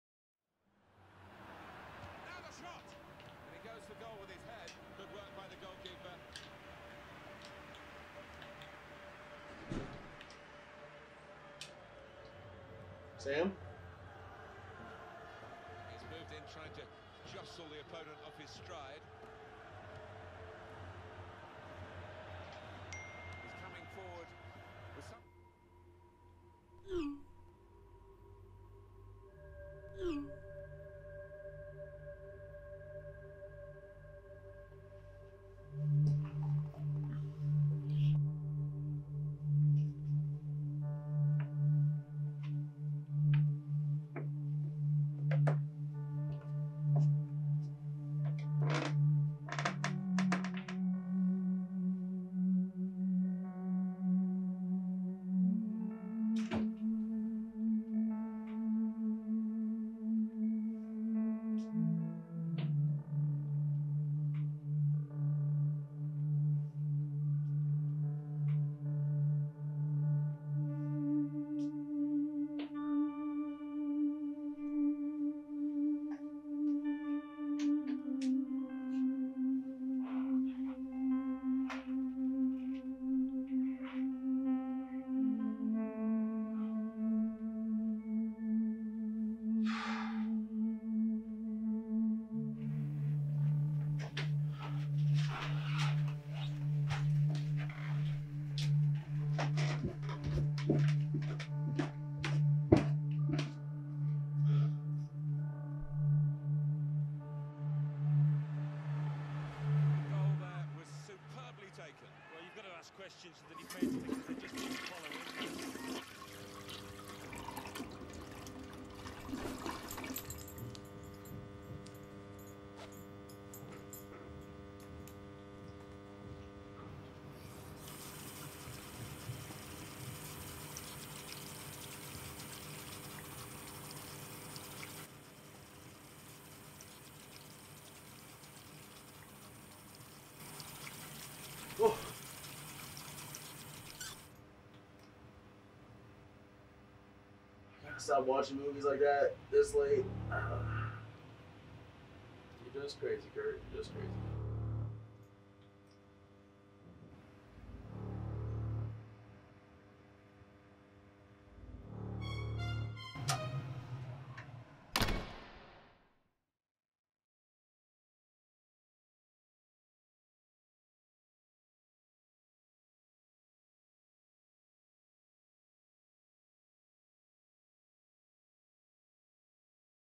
HOME ALONE - Horror Short Film
A man's paranoia blends the line between reality and delusion after a late night of watching a scary movie.